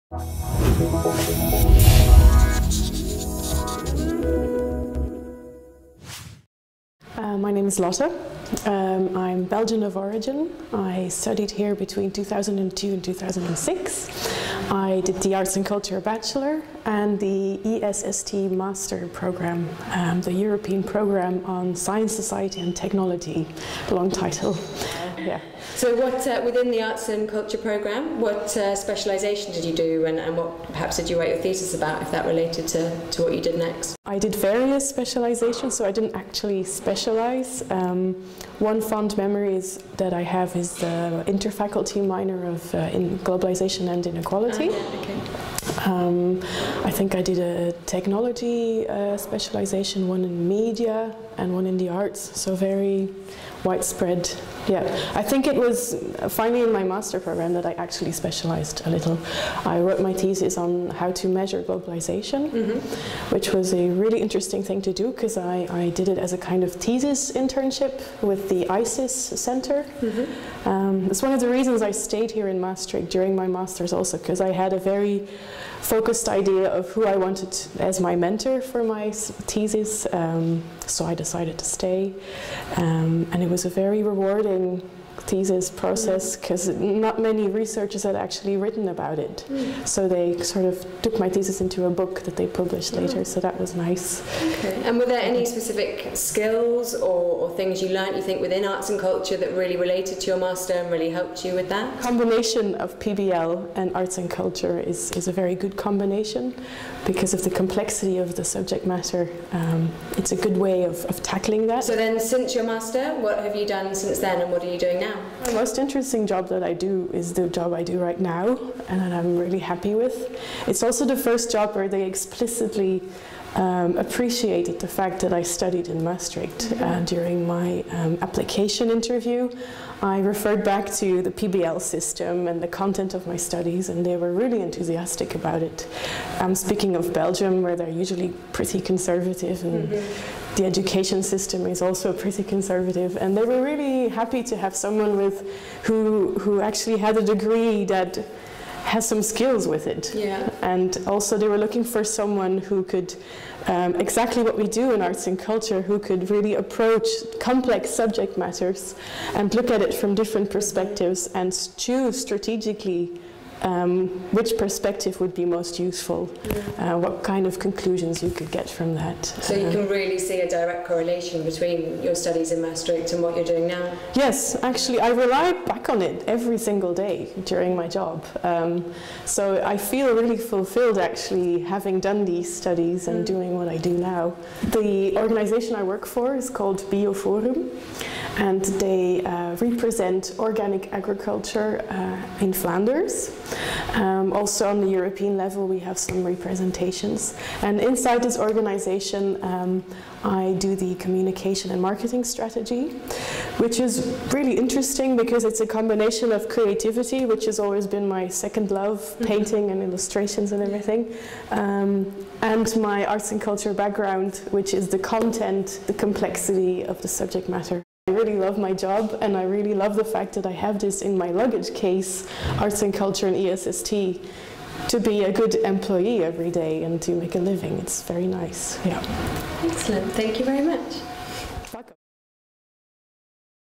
Alumni interview Lotte
In this short video Lotte van Boxem tells you more about her experience at FASoS and how her study helped her in her career.
Lotte studied 'Cultuurwetenschappen' (bachelor's programme) and continued at FASoS with the ESST Masters programme.